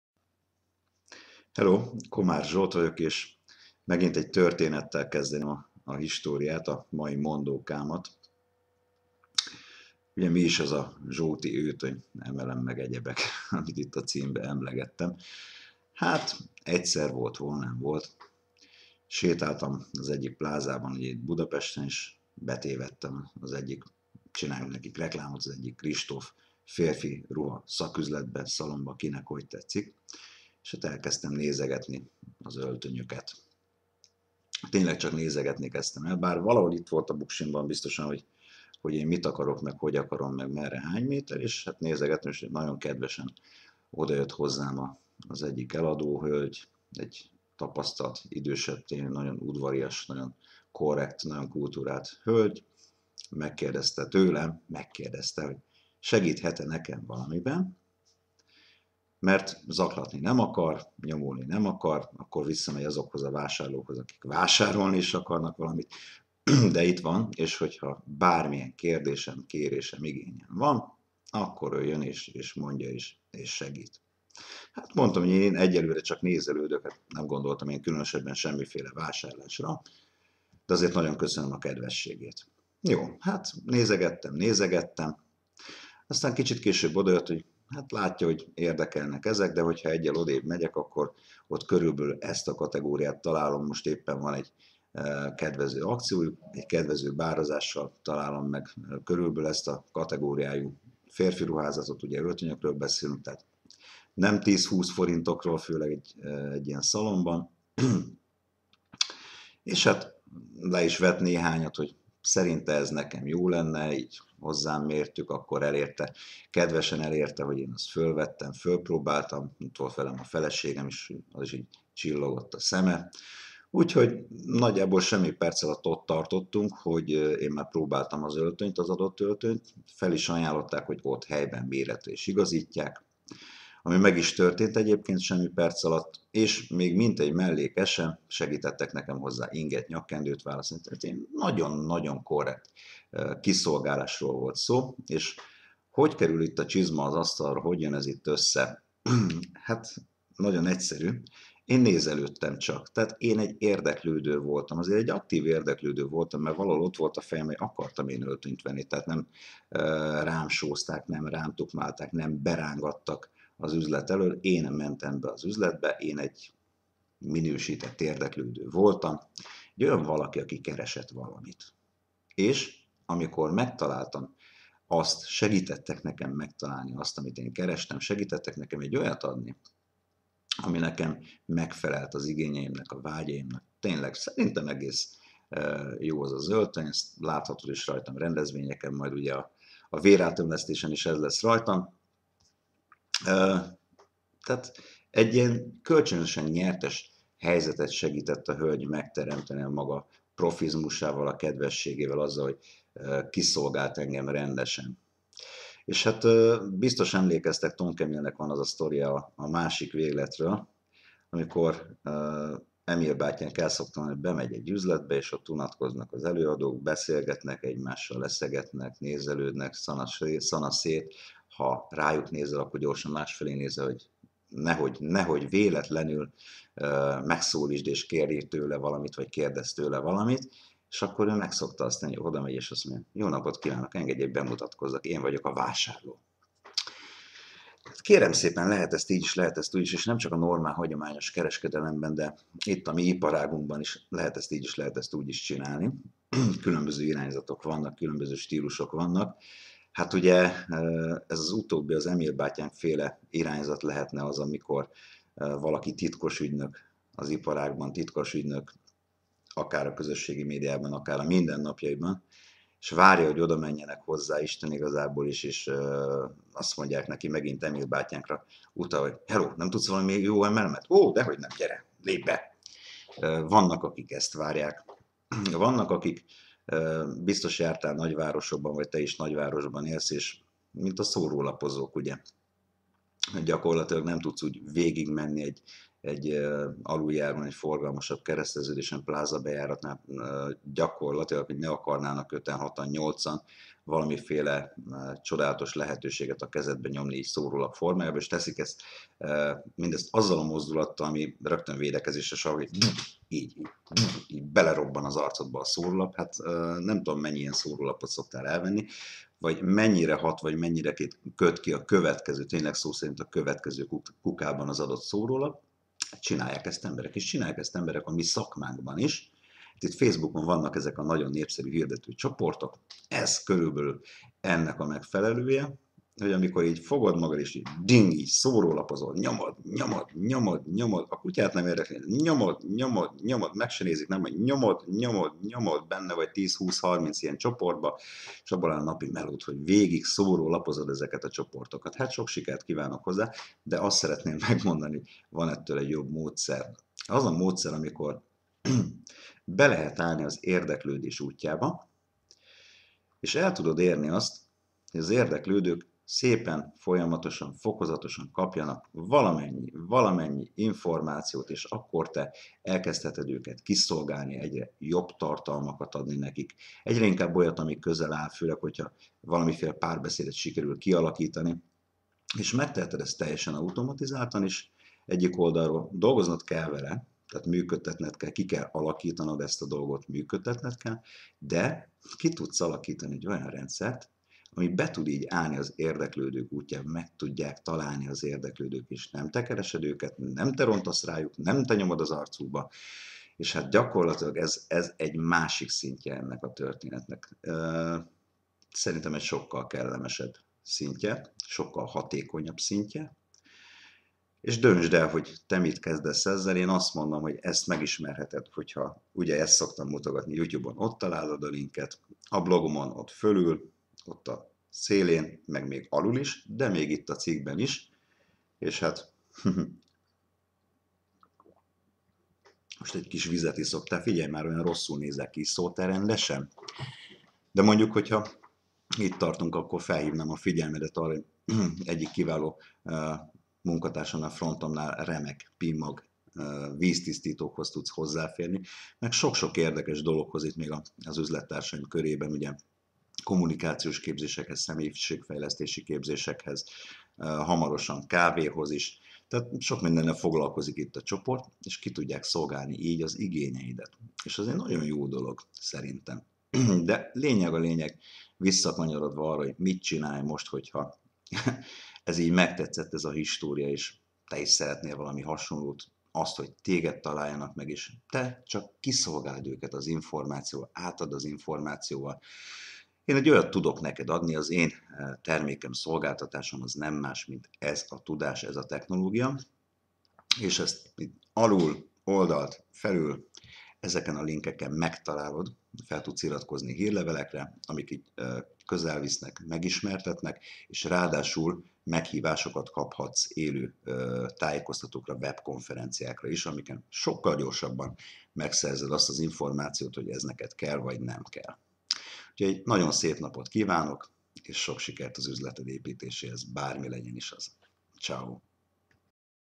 Empower Network Magyarul: Zsóti, Őtttöny, Emelem
Ebben a bejegyzésben is egy tanulságos történetet mesélek el és párhuzamot vonok néhány munkamódszer között. Természetesen van fény az alagút végén! Bővebben a blogomon

Tagok
Empower
Network
Magyarul
Zsóti
Öltöny
MLM
Online
Marketing